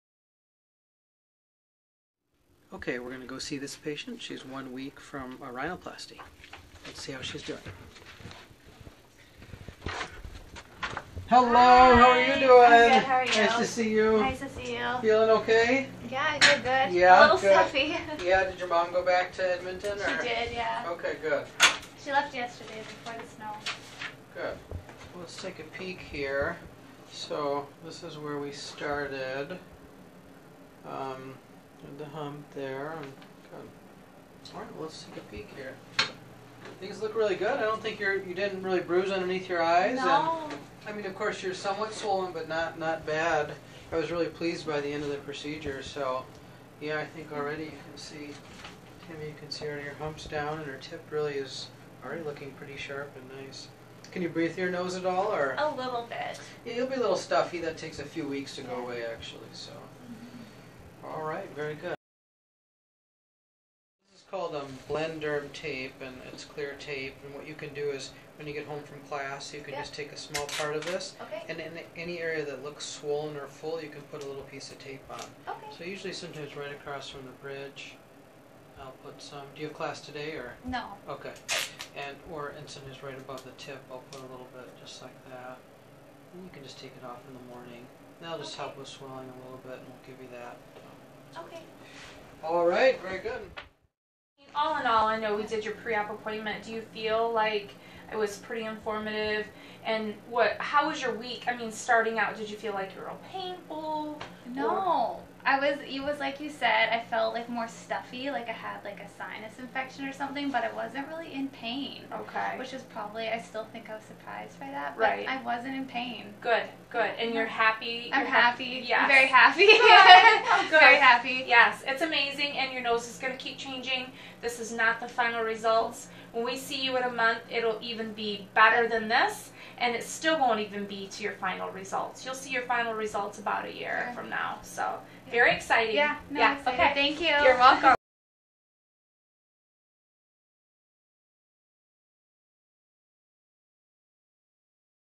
One Week Post-Op Rhinoplasty
Check out this patient's one week post operative appointment and results!